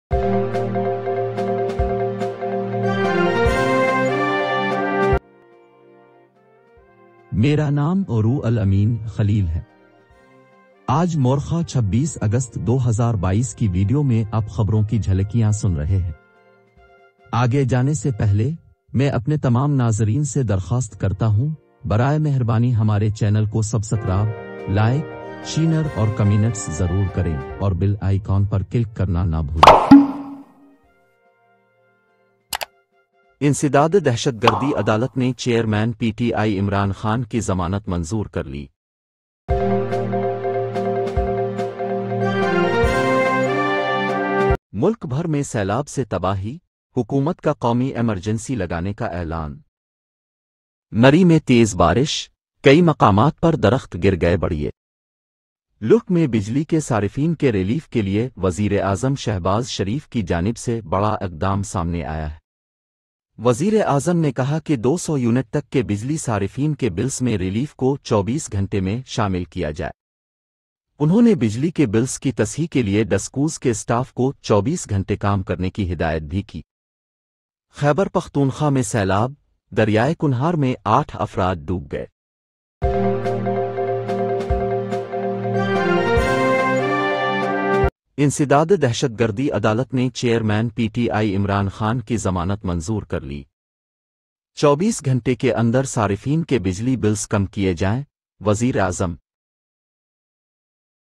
Indus River: High flood alert issued - Pakistan flooding | News Bulletin at 8 AM | PDMA Weather
23,113 views  Aug 26, 2022  Indus River: High flood alert issued - Pakistan flooding | Geo News
دریائے سندھ: سیلاب کا ہائی الرٹ جاری